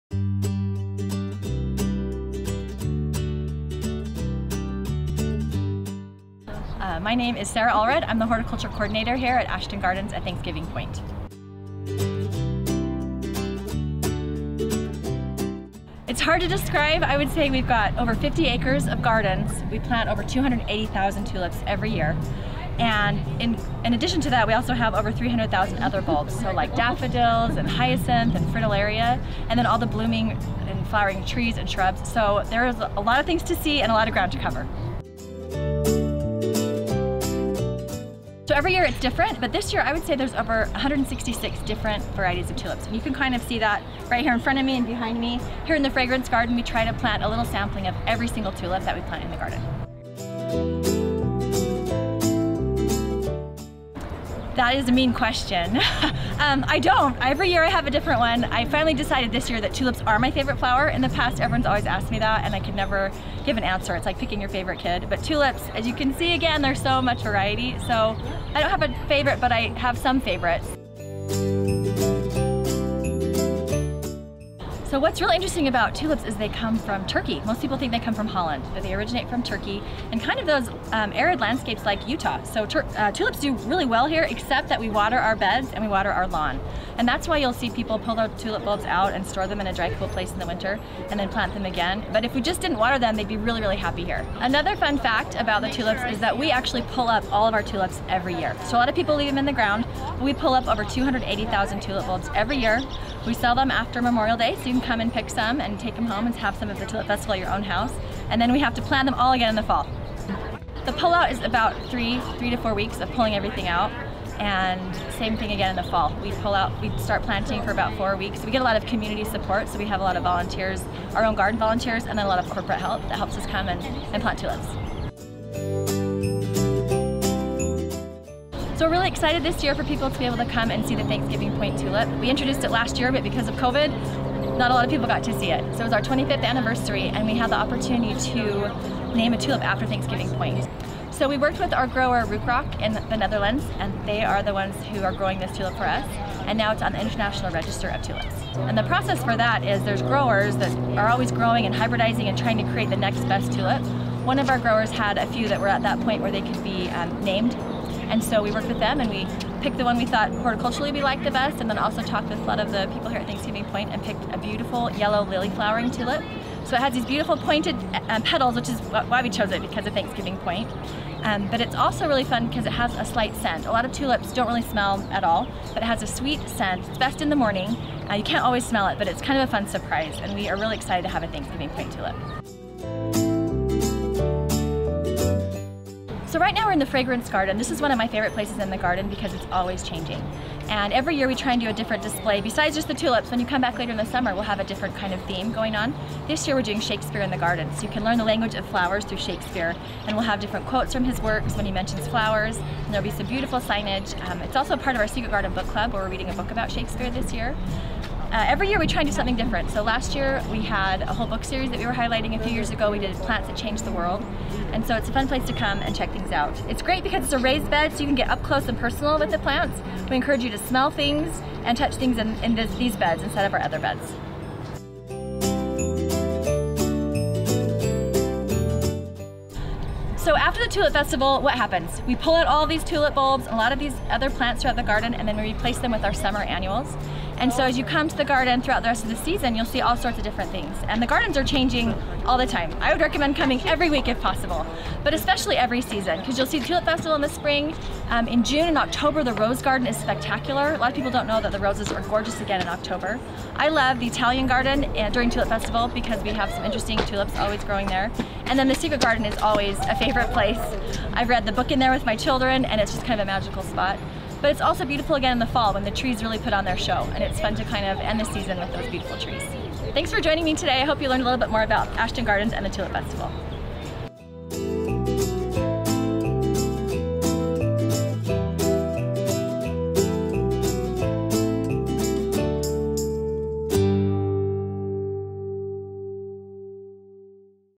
Behind the Scenes | Tulip Festival
Sarah meets us in the Fragrance Garden to share some details of how the Tulip Festival happens, her favorite flowers this year, and an up close look at the special Thanksgiving Point Tulip!